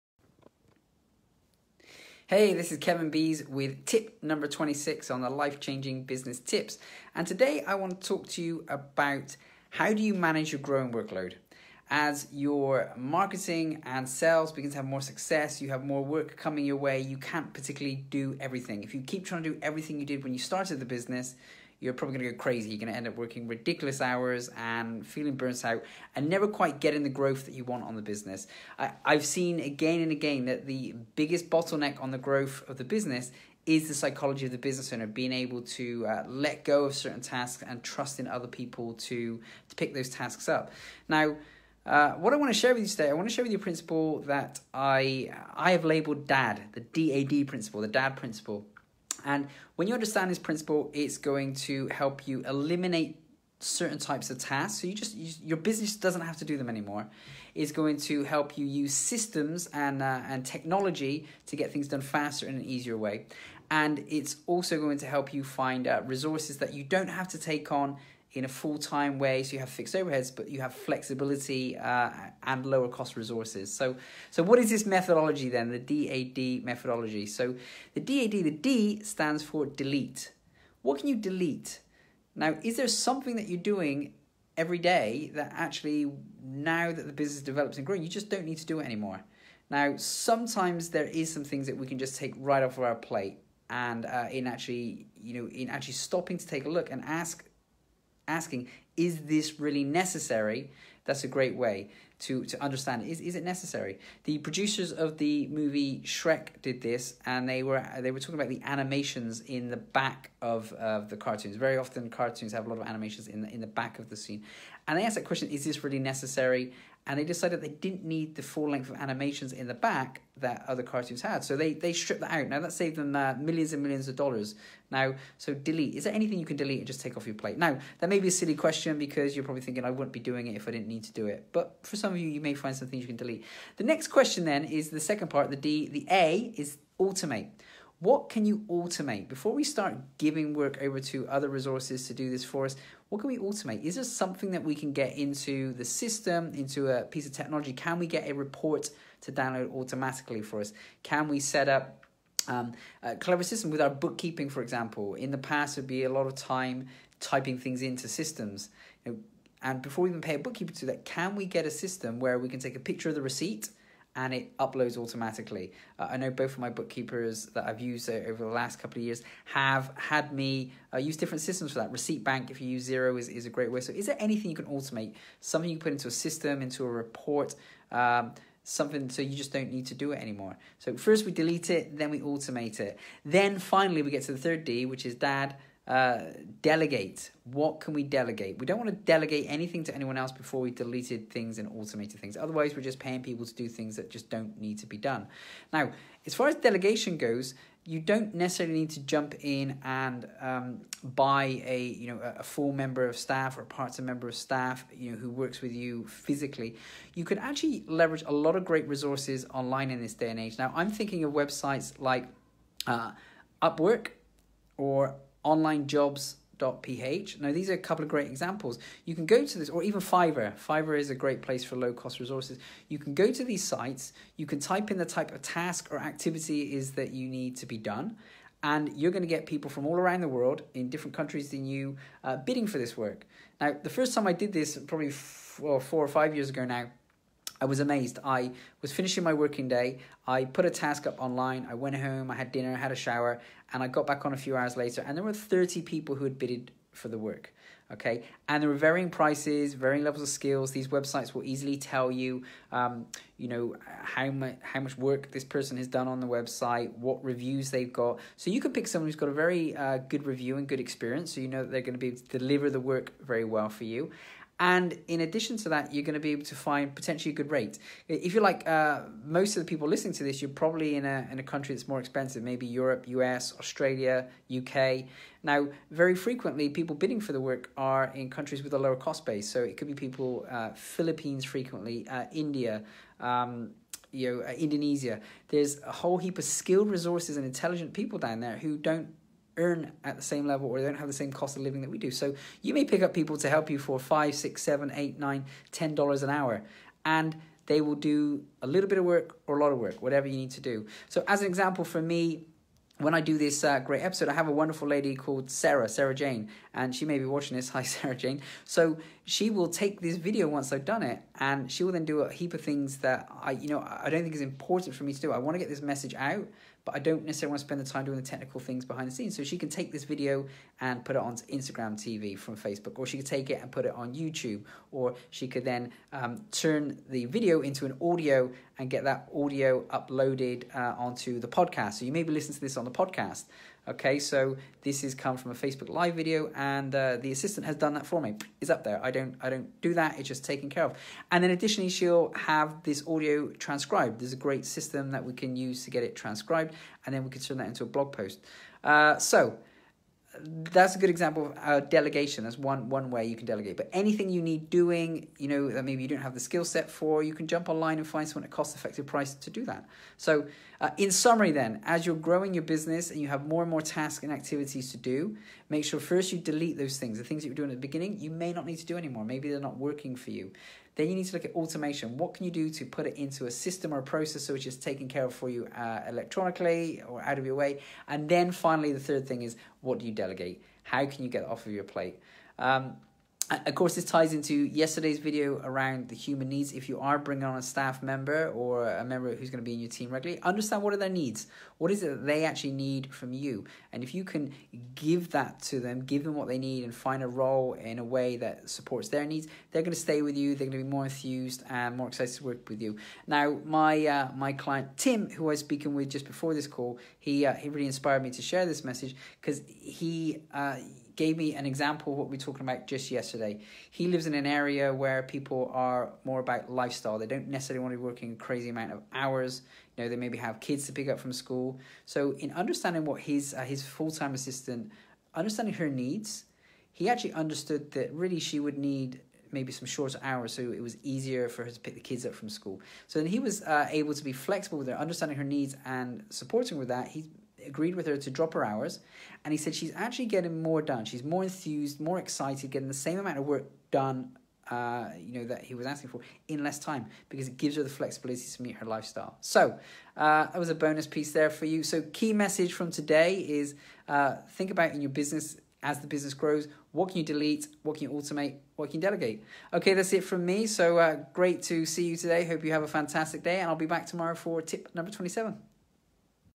How To Manage Your Growing Workload - Life Changing Business Tips #26
Business Growth Expert Kevin Bees is sharing Life Changing Business Tips.

Today,  As your business grows, the work required grows, and there comes a point where you need to STOP doing certain tasks, or you become the bottleneck in the business growth potential.

Before you rush out and buy expensive staff and commit yourself to fixed costs - consider using the “DAD” methodology.

The DAD methodology will help you
- eliminate tasks - leverage technology
- help you find low cost and flexible resources to do the work for you

Listen in to find out HOW you can do this!